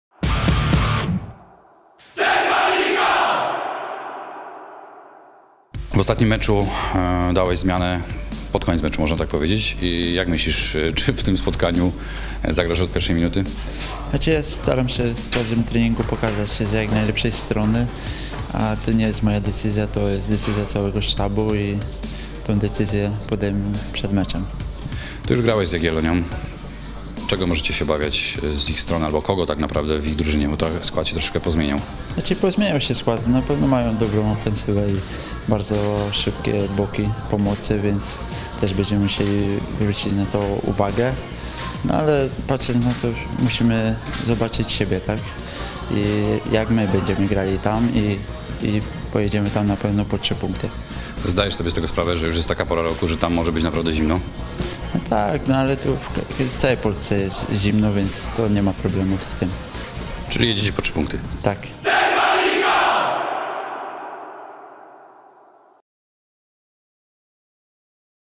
Patryk Fryc przed meczem w Białymstoku
Wywiad podczas treningu